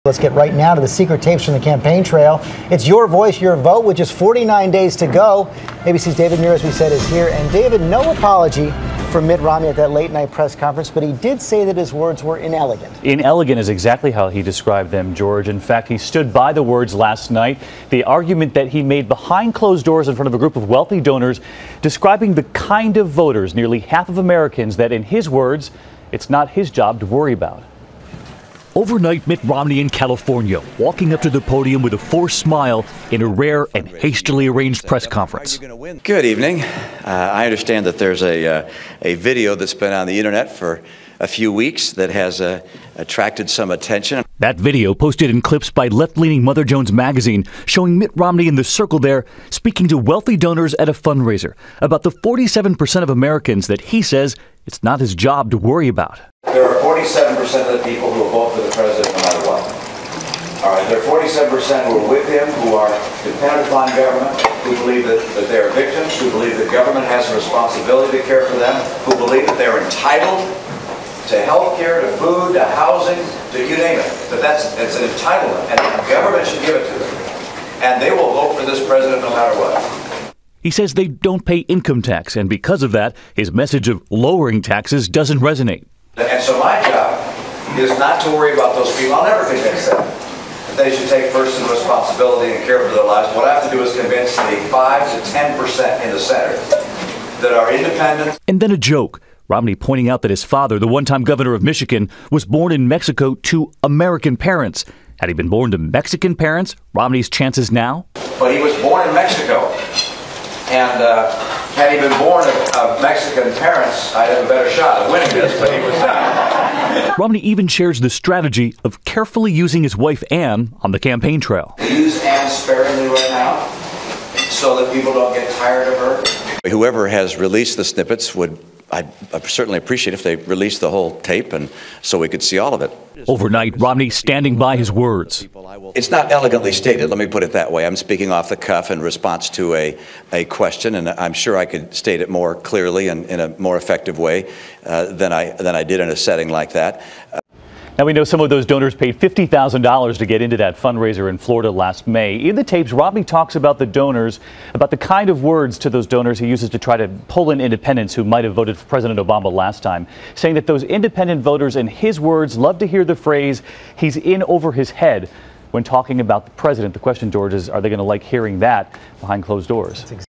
Mitt Romney Fundraising Video: Candidate's 'Inelegant' Words in Boca Raton Caught on Tape
The Republican candidate is seen on camera discussing 'entitled' voters at a fundraiser.